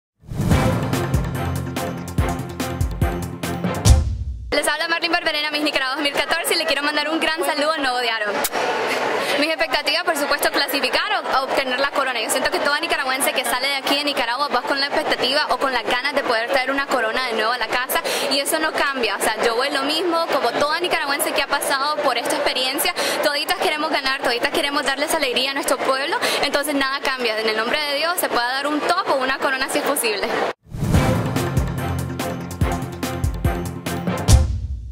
Marline Barberena comparte sus expectativas
Antes de partir a  Miss Universo Marline nos dejó clara sus metas. ANDREA SEPULVEDA / END